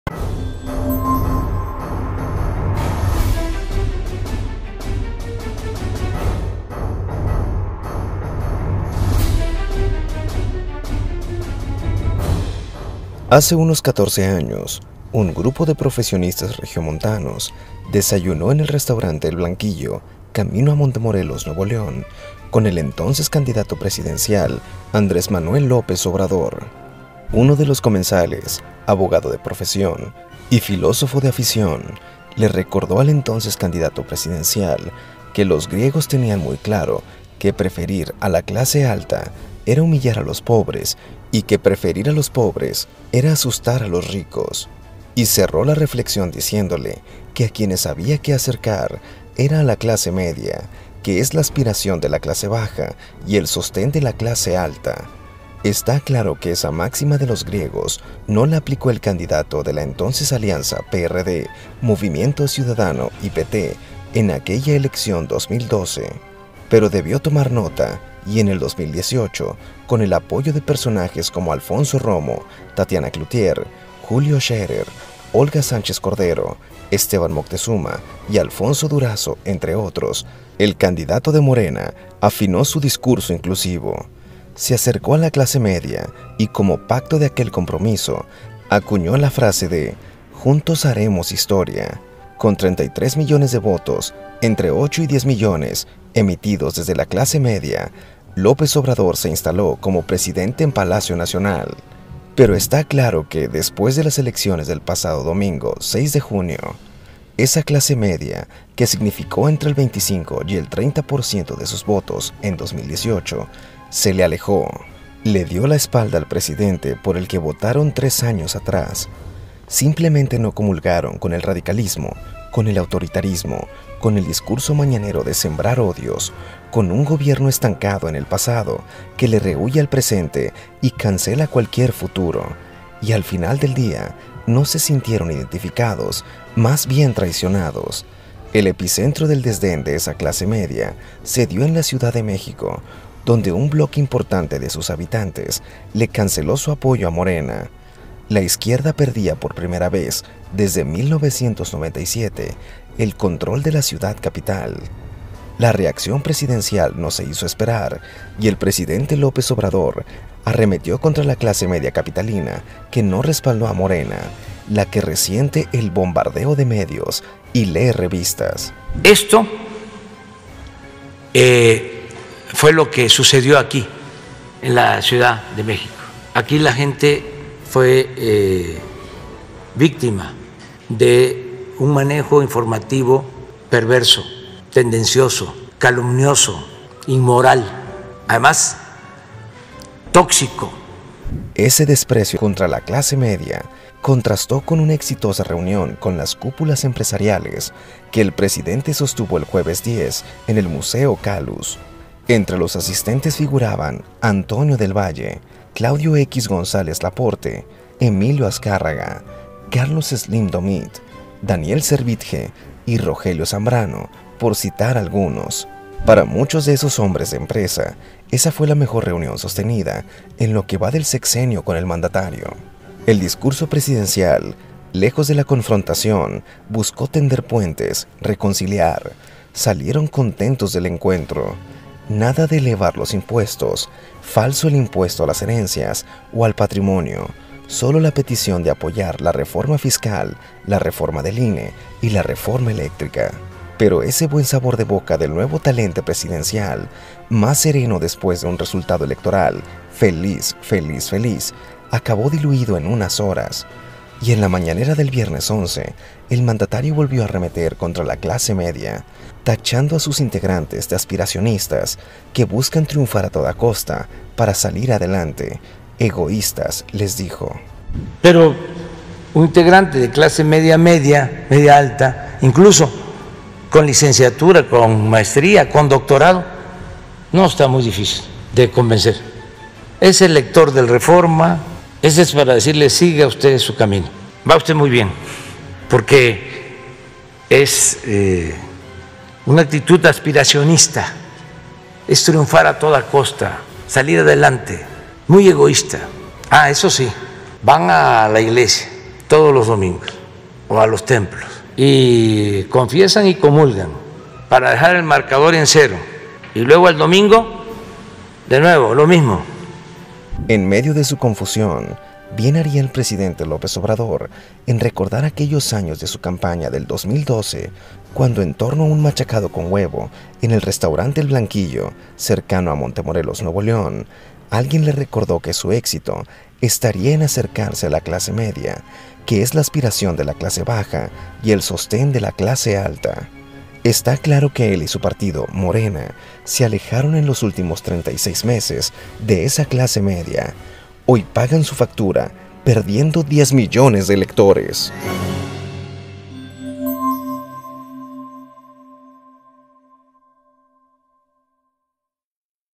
La “maldita” clase media
En medio de su confusión, bien haría el presidente López Obrador en recordar aquellos años de su campaña del 2012, cuando alguien le recordó que su éxito estaría en acercarse a la clase media, que es la aspiración de la clase baja y el sostén de la clase alta.

👉La opinión de Ramón Alberto Garza en QueAlguienMeExplique